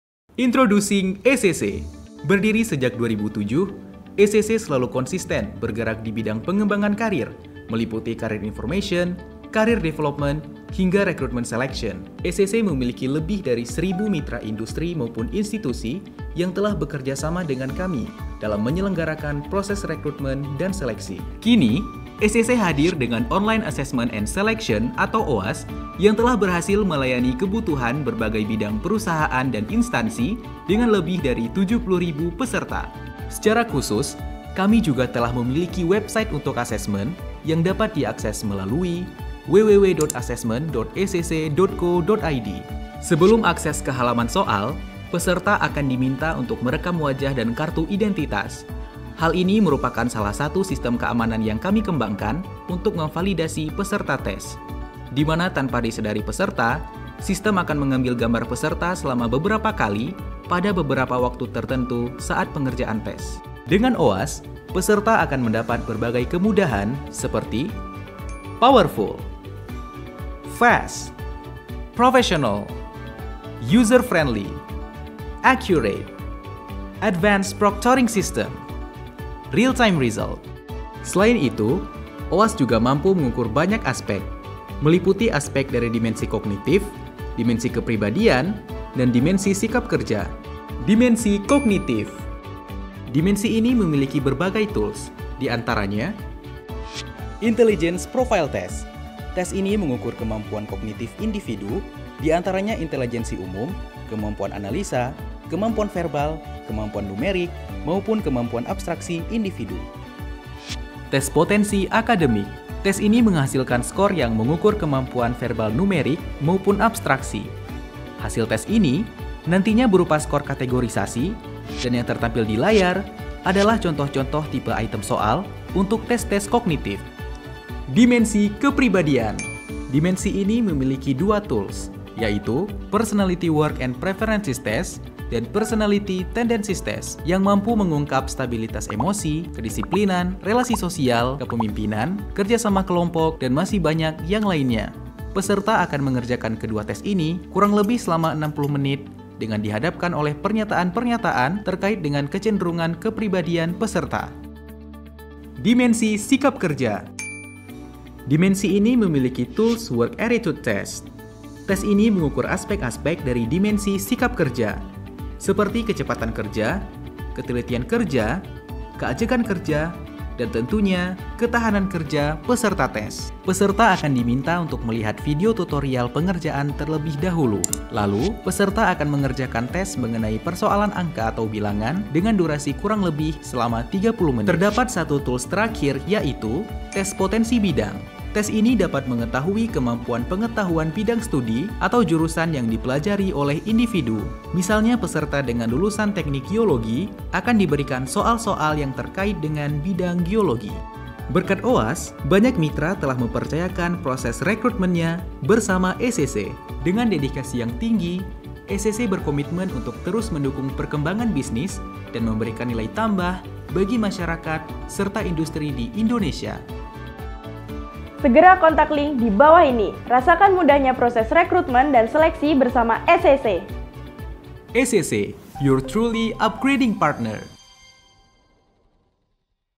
ECC menghadirkan layanan Online Assessment & Selection (OAS)
OAS ECC merupakan layanan asesmen yang dilaksanakan secara daring (online) untuk memenuhi kebutuhan industri maupun instansi dalam melihat potensi kandidat baik dalam proses seleksi calon karyawan (recruitment & selection) maupun pemetaan potensi karyawan (potential review).
OAS ECC dapat dilaksanakan tanpa melihat jumlah peserta, berapapun jumlah peserta, ECC siap menjadi partner asesmen bapak / ibu. Selain itu, ada beberapa faktor yang bisa menjadi bahan pertimbangan Bapak/Ibu untuk memilih OAS ECC diantaranya: OAS ECC itu Powerfull, Professional, Accurate, Fast, User Friendly, Real Time, serta dilengkapi dengan Advance Proctoring System.

Instansi anda tertarik?, atau masih perlu diskusi lebih lanjut,
Jangan sungkan untuk menghubungi kami: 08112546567